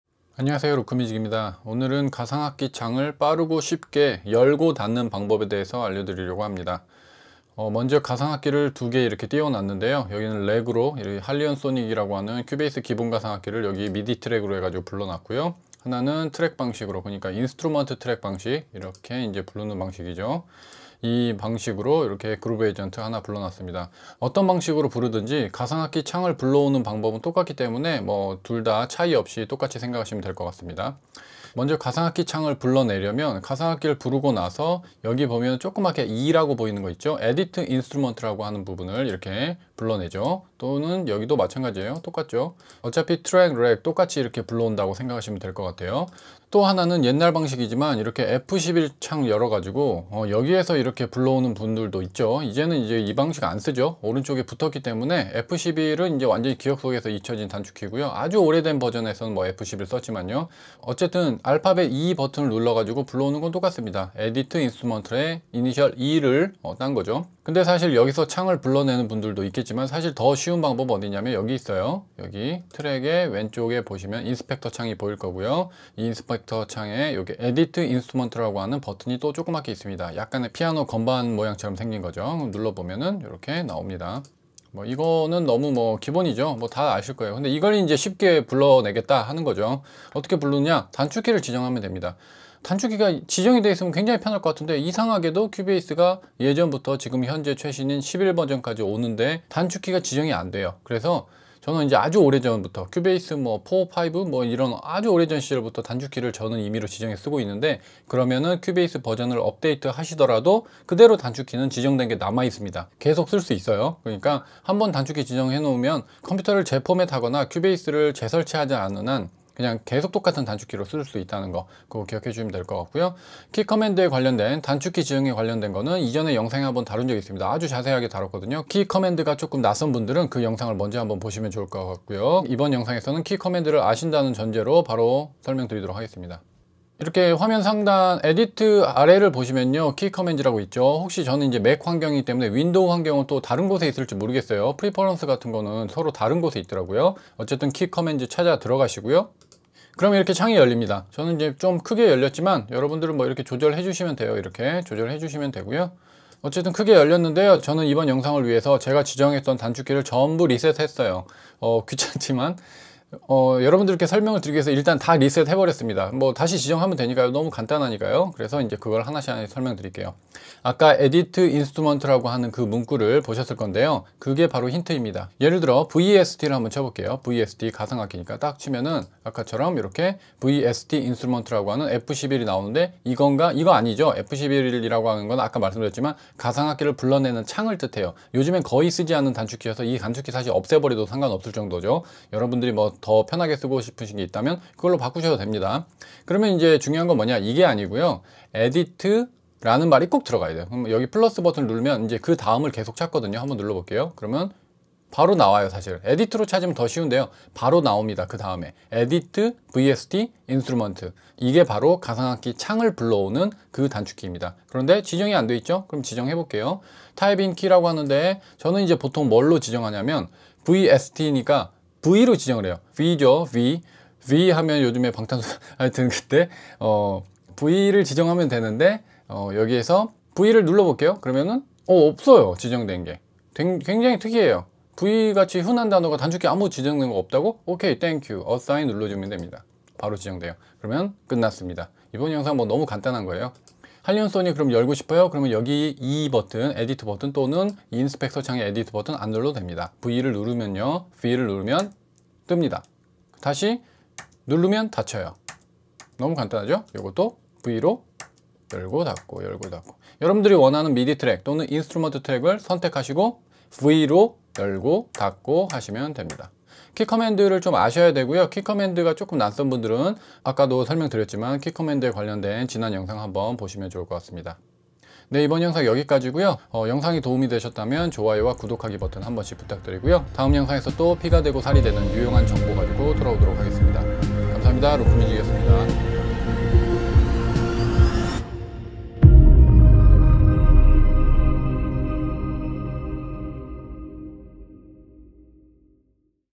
[큐베이스 꿀팁] 불러놓은 가상악기 창 쉽고 빠르게 열고 닫는 법 | 단축키 지정 | Edit VST Instrument
알아두면 쓸모있는 큐베이스 프로 팁 #67 - 불러놓은 가상악기 창 쉽고 빠르게 열고 닫는 법 | 단축키 지정 | Edit VST Instrument

: 큐베이스에서 가상악기를 Rack 방식의 미디 트랙이나 Track 방식의 인스트루먼트 트랙으로 불러놓고, 그 가상악기 안의 음원을 바꾸거나 에디팅 하려고 다시 창을 띄우려면 보통 e 버튼을 마우스로 클릭해서 열고 닫고 해야 합니다.

가상악기 창을 열고 닫는 경우는 작업하면서 정말 자주 있는 일이기 때문에 이 기능이 단축키로 이미 지정되고도 남았을만 한데, 2021년 최신 버젼까지도 아직 기본 단축키로 지정 되어 있지 않습니다. 그렇다면 사용자가 간단하게 임의로 단축키를 지정해서 마우스 클릭을 쓰지 않고 정말 간편하게 창을 열고 닫을 수 있습니다.

이번 영상에서는 그 불러놓은 가상악기 창을 쉽고 빠르게 열고 닫을 수 있는 방법, 단축키 지정에 대해 알아봅니다.

@yamahamusickorea (한국 큐베이스 Distributor)